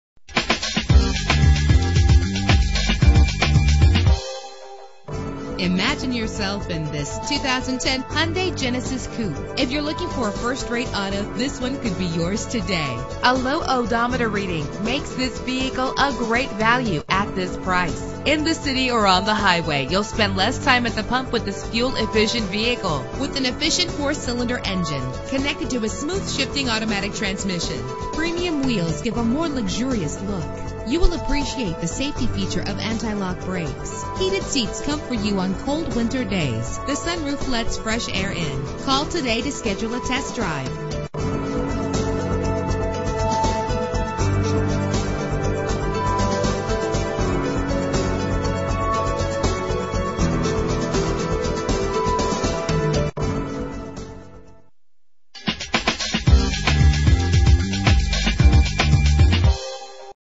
2010 Hyundai Genesis Coupe - Ottawa ON
Year: 2010
Make: Hyundai
Model: Genesis Coupe
Trim: 0T
Engine: 2.0 liter inline 4 cylinder 16 valve
Transmission: Automatic
Color: Black
Mileage: 22620
Address:
2788 Bank Street
Ottawa, ON K1T 1N3